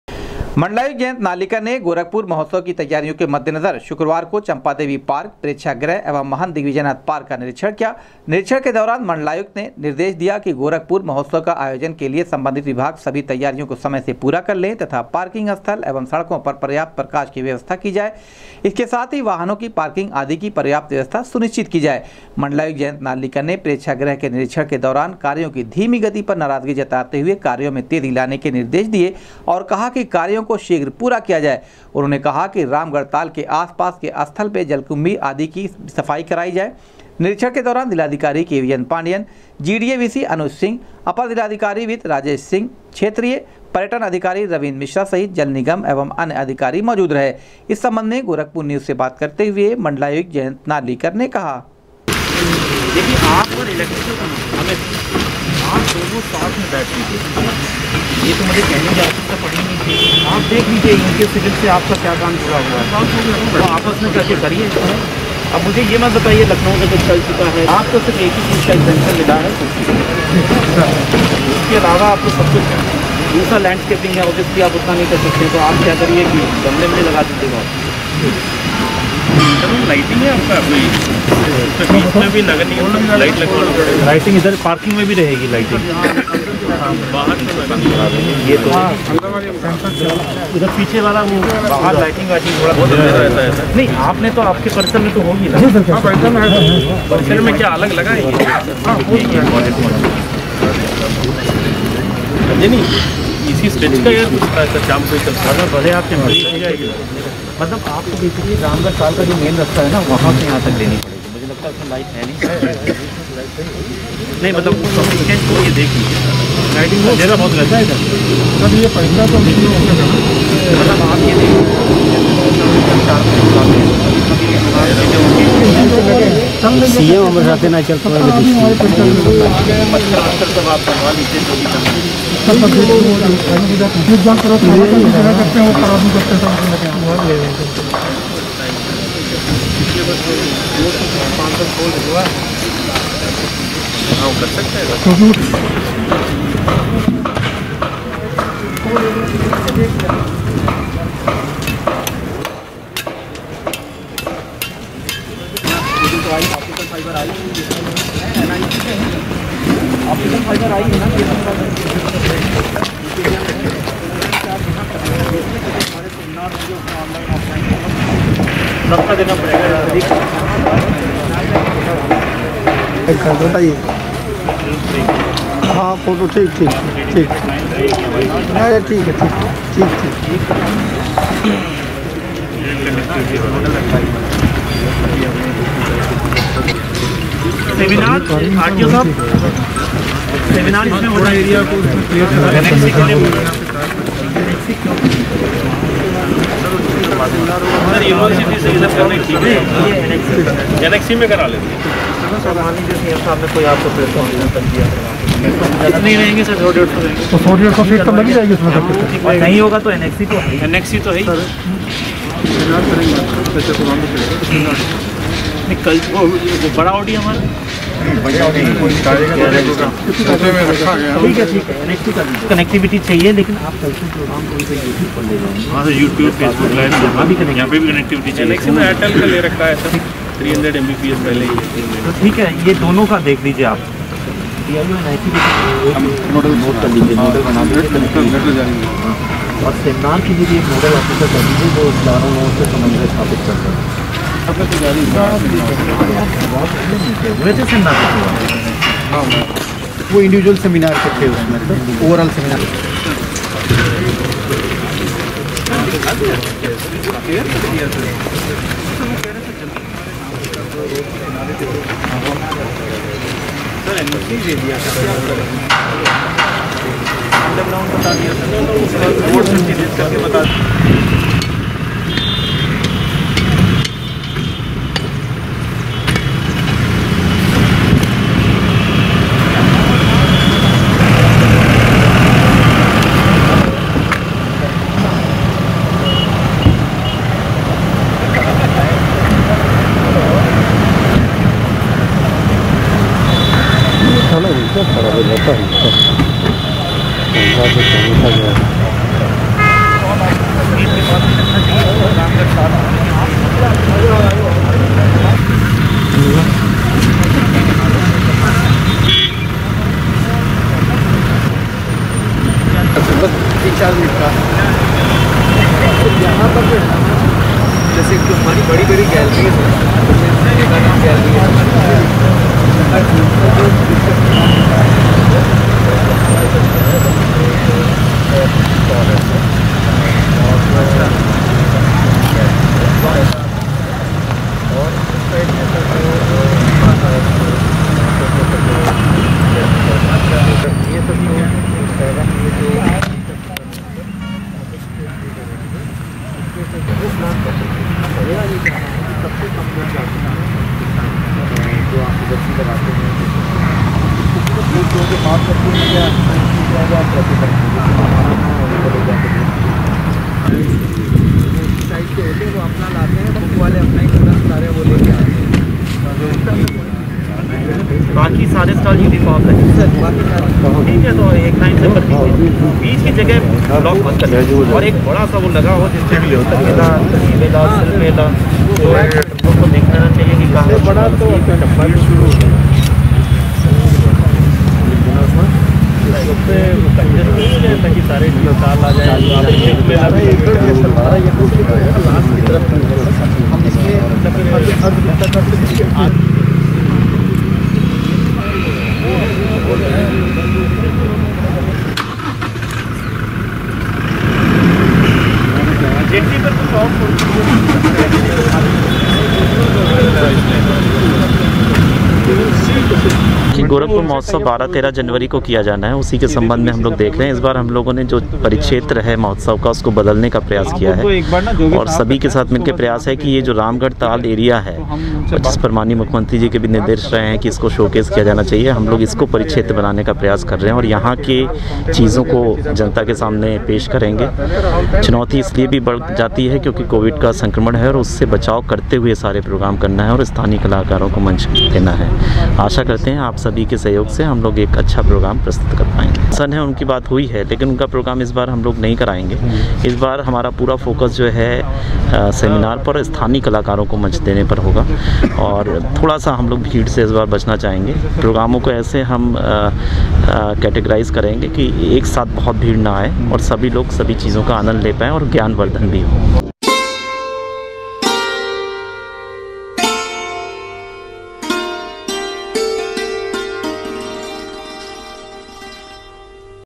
गोरखपुर महोत्सव के तैयारियो के मद्देनजर मण्डलायुक्त ने सम्बंन्धित अधिकारियो के साथ किया चम्पा देवी
गोरखपुर महोत्सव के तैयारियो के मद्देनजर मण्डलायुक्त ने सम्बंन्धित अधिकारियो के साथ किया चम्पा देवी पार्क, प्रेक्षागृह एवं महन्त दिग्विजयनाथ पार्क का निरीक्षण,कहा,महोत्सव की सभी तैयारियां समय से पूरी कर ले सम्बन्धित विभाग